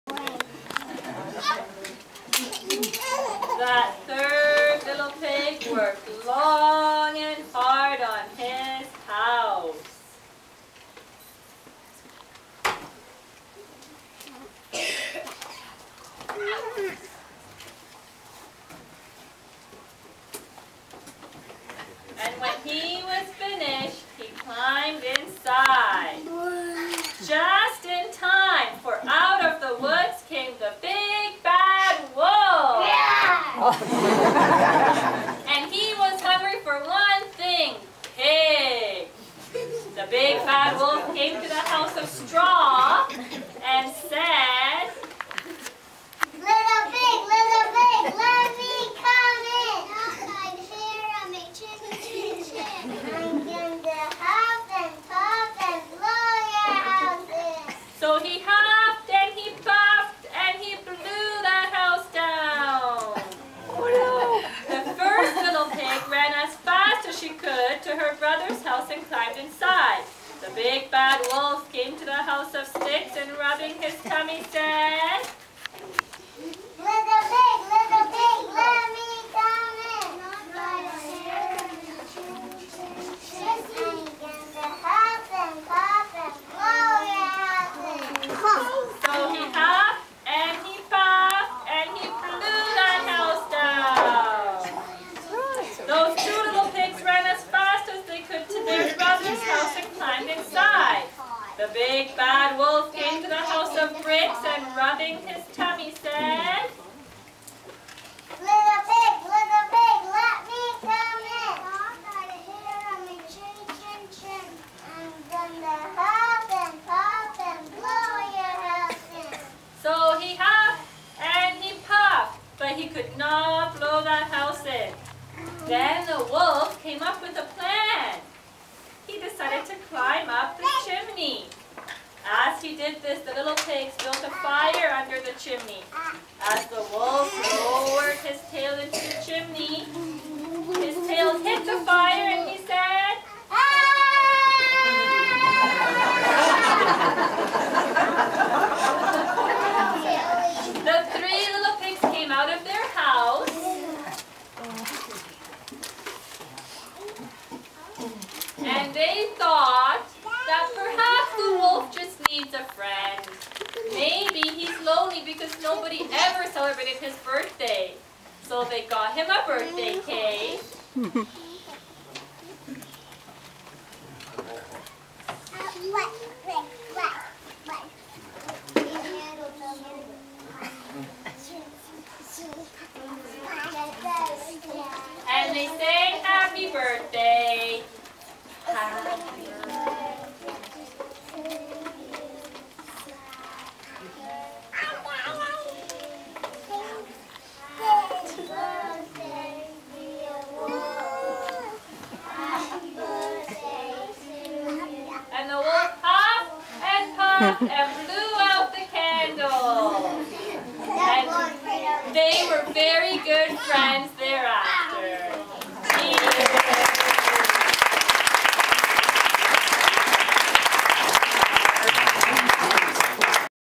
The Three Pigs
Gloria's preschool graduation play